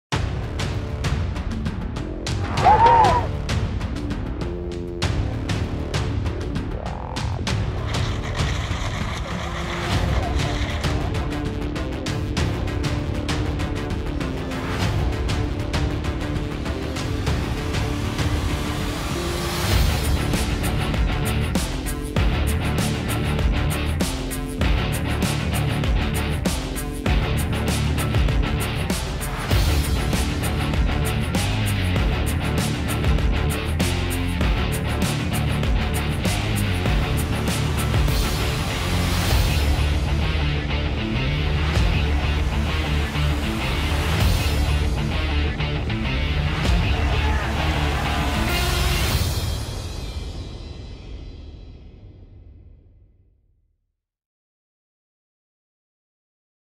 Who to Watch | Men's Giant Slalom | World Cup Opening | Sölden | FIS Alpine
Find out some of the best Men's Giant slalom specialists to look out for in the 2020/21 World Cup opening.